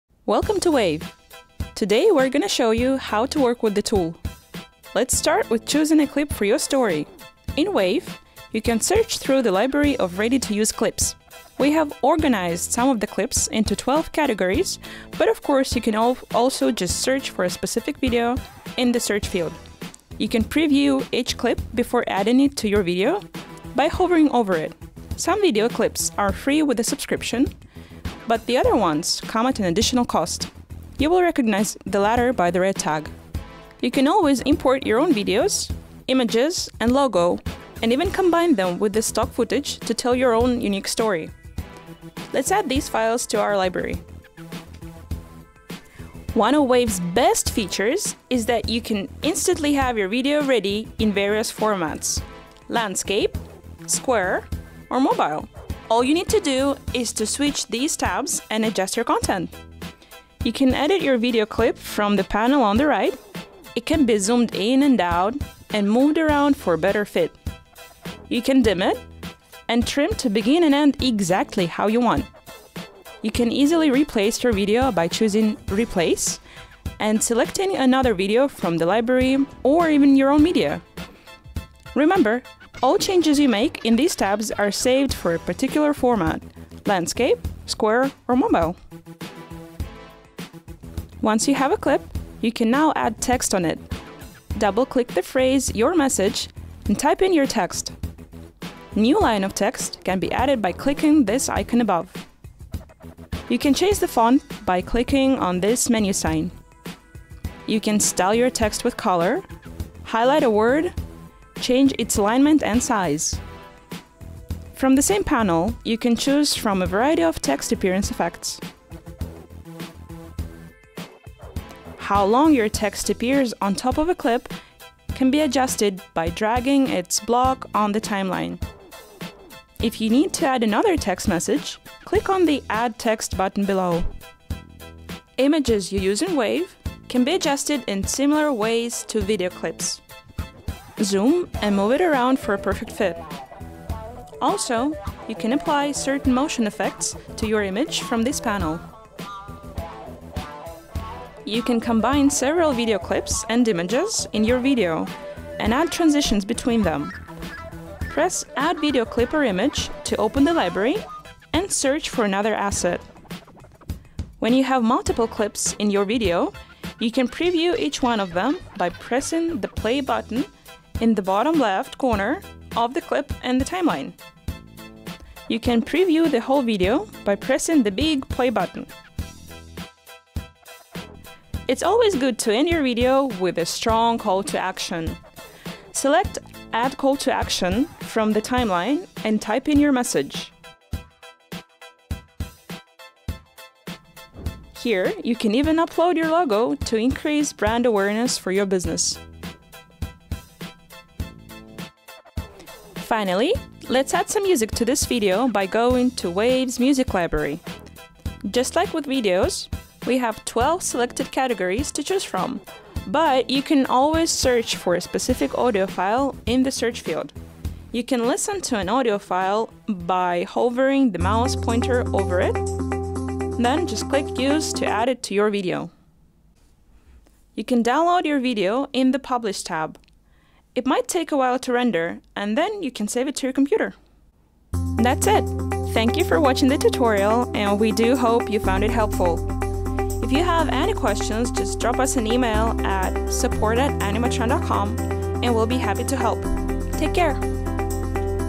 Wave.video Walkthrough Tutorial
Learn your way around Wave.video with this short tutorial. After watching this video, you will be able to start working in the video maker right away!